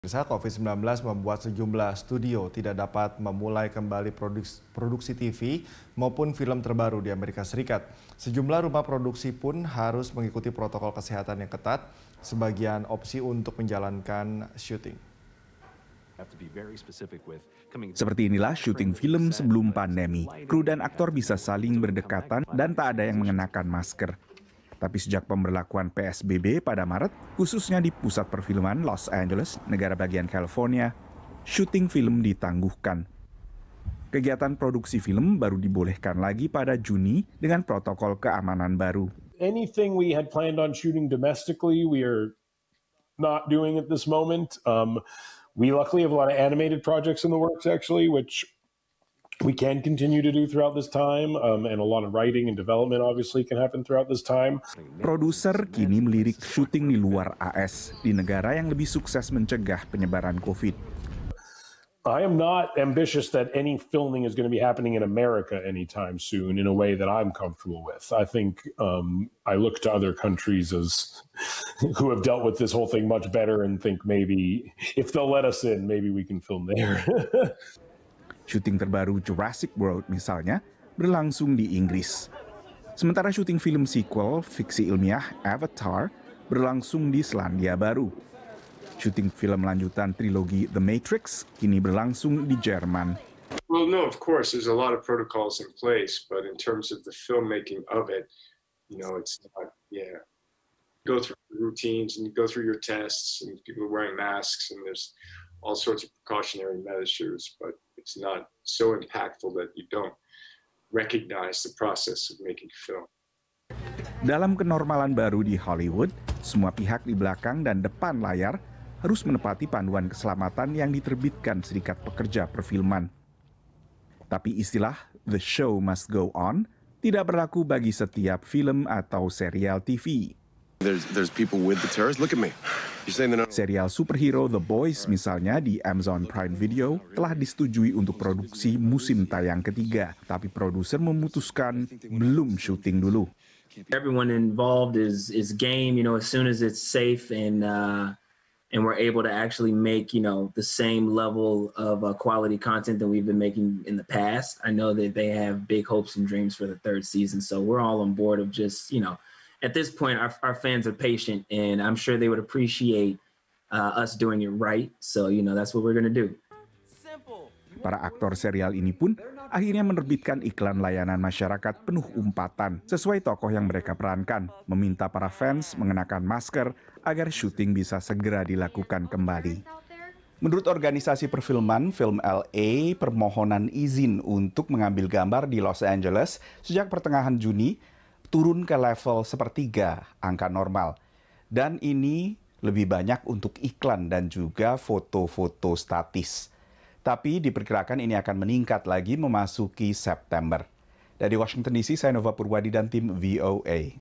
Industri Film Amerika Serikat di Tengah Pandemi Covid-19
Covid-19 membuat sejumlah studio tidak dapat memulai kembali produksi TV maupun film terbaru di Amerika Serikat. Sejumlah rumah produksi harus mengikuti protokol kesehatan yang ketat sebagian opsi untuk menjalankan syuting.